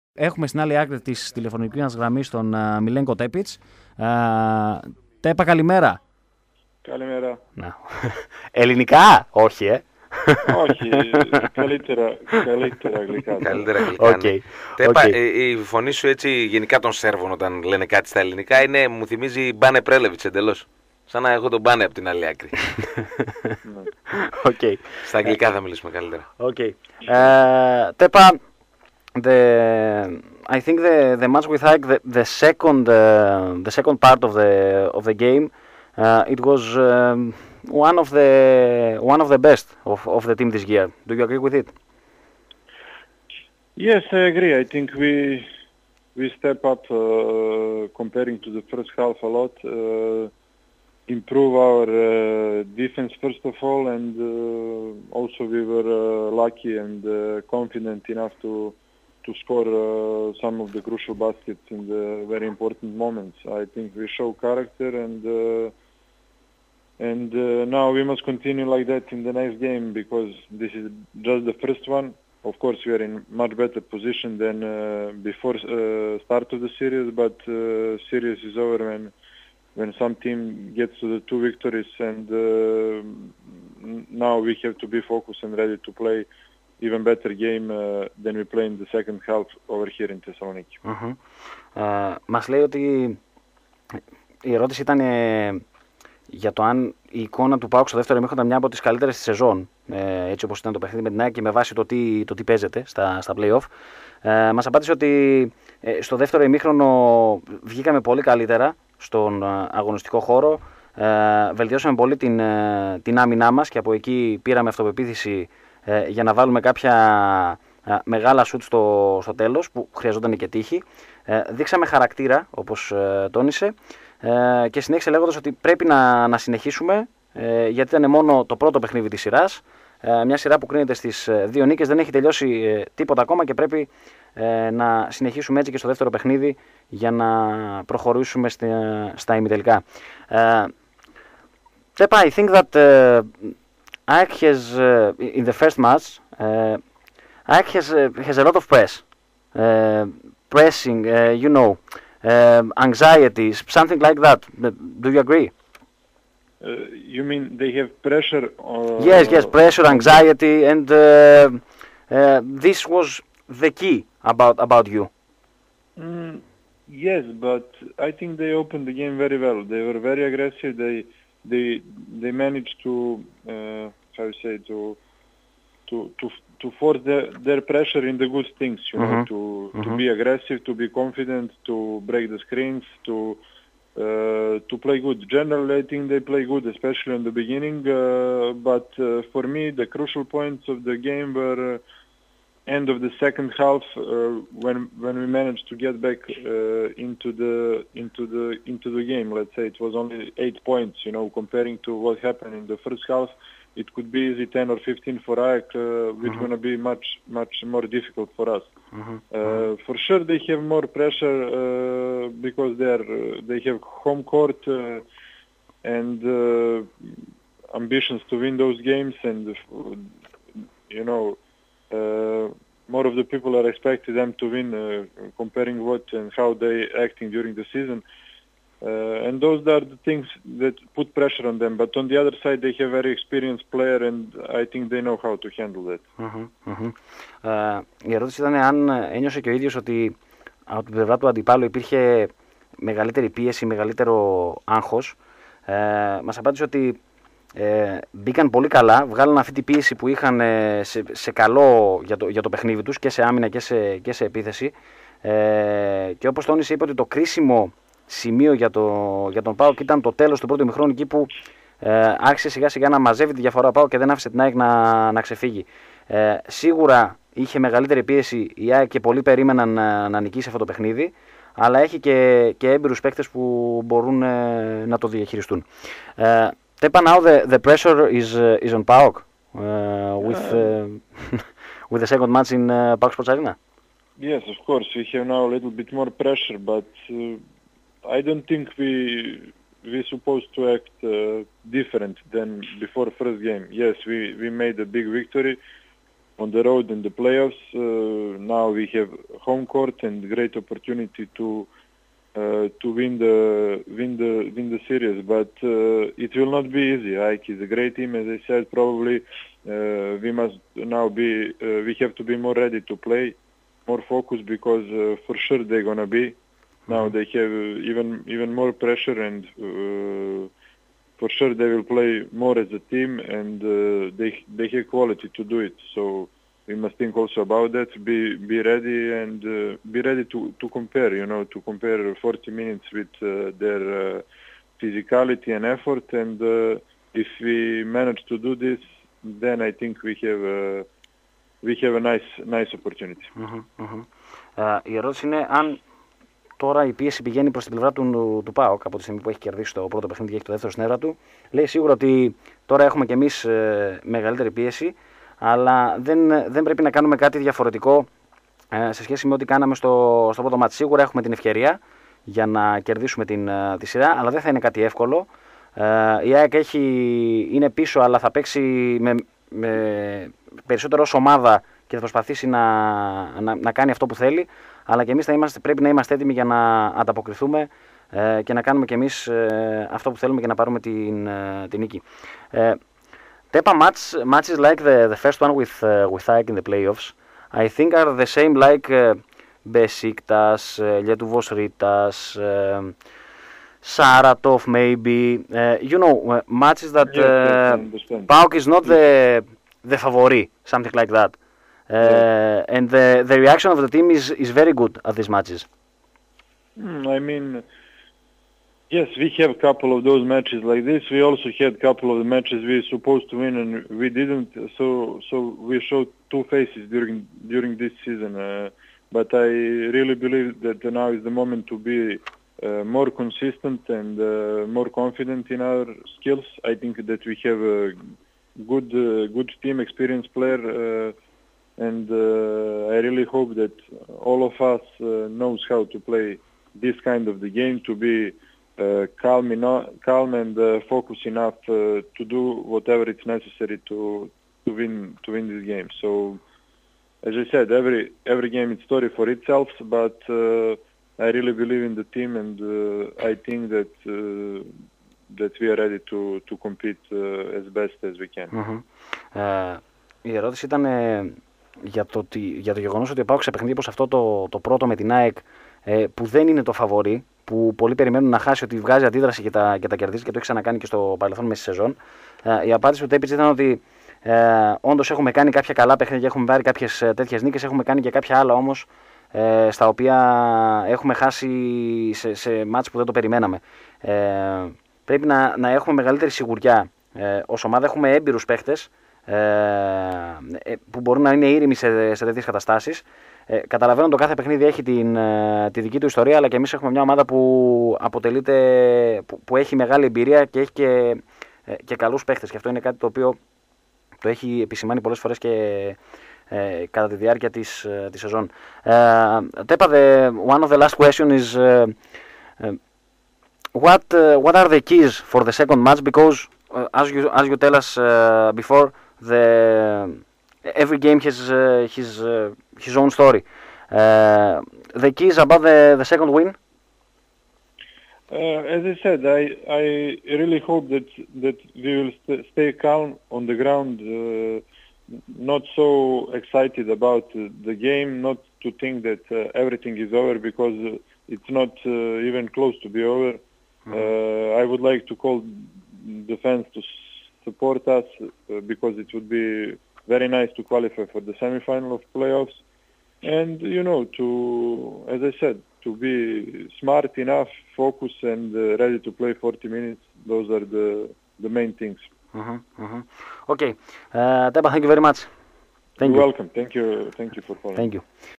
Μιλένκο Τέπιτς: "Δεν έχει τελειώσει τίποτα..."
Παραμένει προσγειωμένος ο Μιλένκο Τέπιτς. Ο Σέρβος μίλησε στο "PAOK24" του "Libero107,4" για το 1-0, το παιχνίδι του Σαββάτου και τον κόσμο.